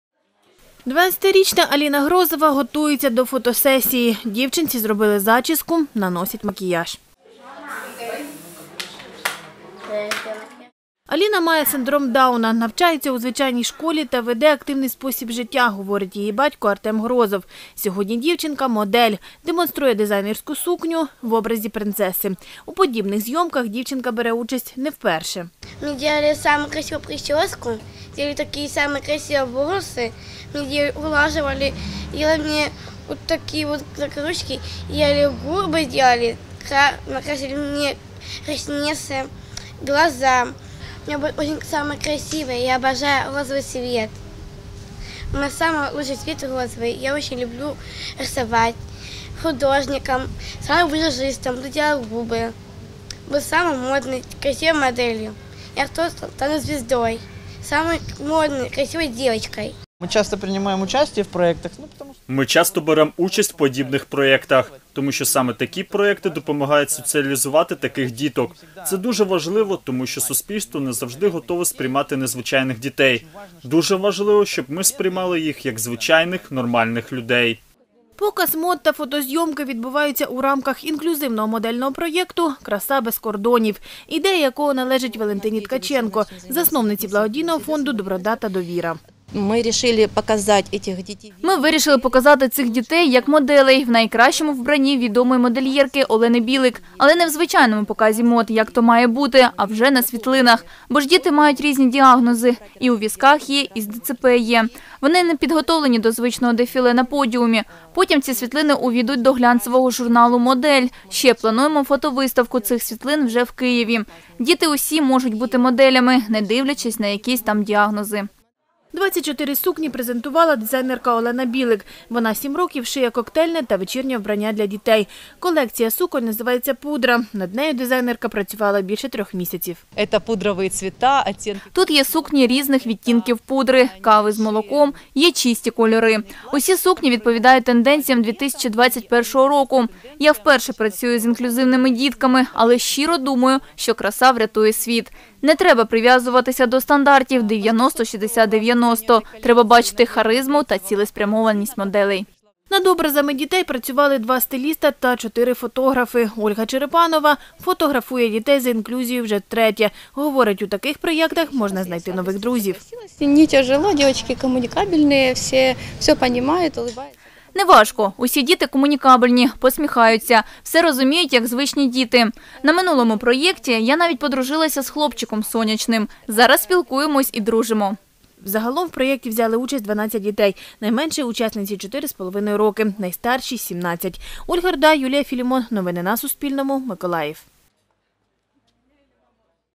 "Краса без кордонів": у Миколаєві відбувся дитячий інклюзивний показ мод
11 червня в Миколаєві відбувся дитячий інклюзивний показ мод "Краса без кордонів". Моделей фотографували у дизайнерських сукнях для глянцевого журналу. Загалом в проєкті взяли участь 12 дітей. Найменшій учасниці чотири роки, найстаршій ‒ 17.

Авторка: Ольга Руда, операторка: Юлія Філімон.